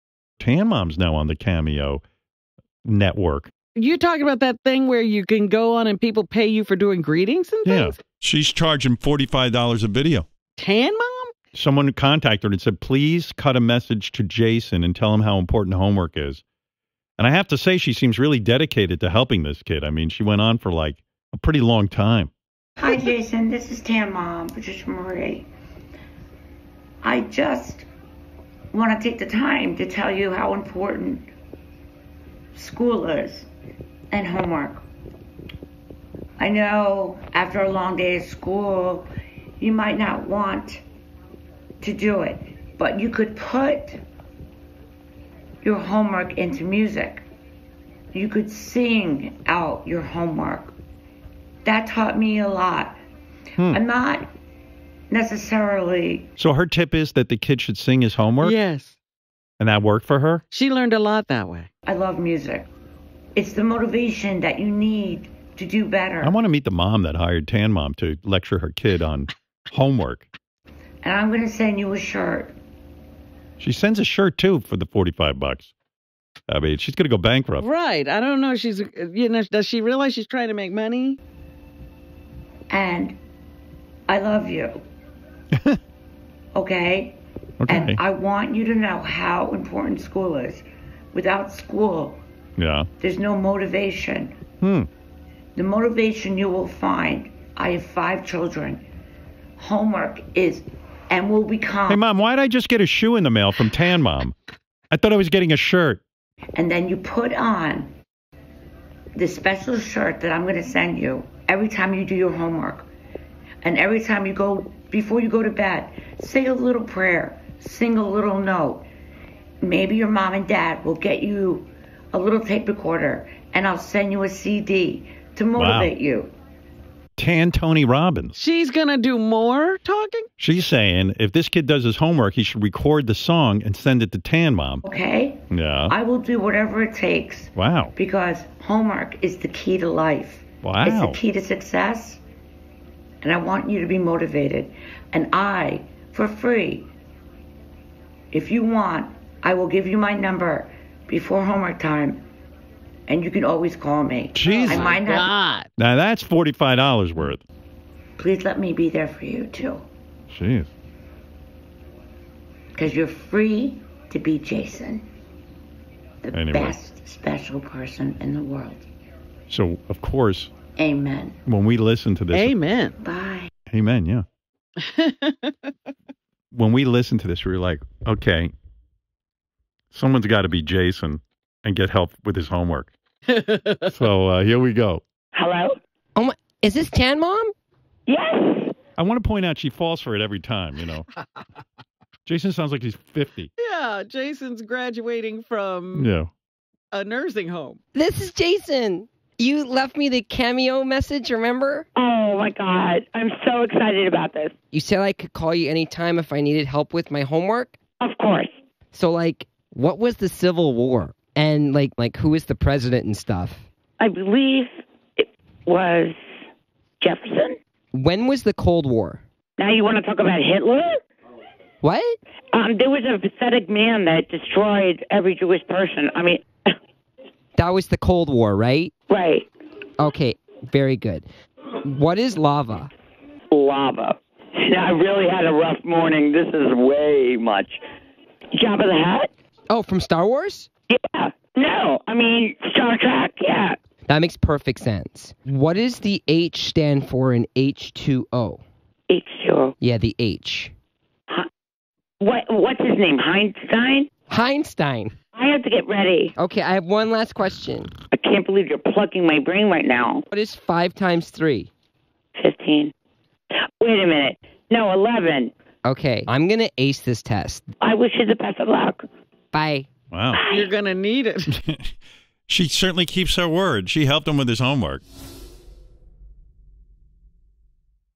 Tan Mom Offers Homework Help to a Young Fan on Cameo
Tan Mom is the latest Wack Packer to join Cameo, the video shout-out service where fans can pay money for personalized messages from their favorite celebrities.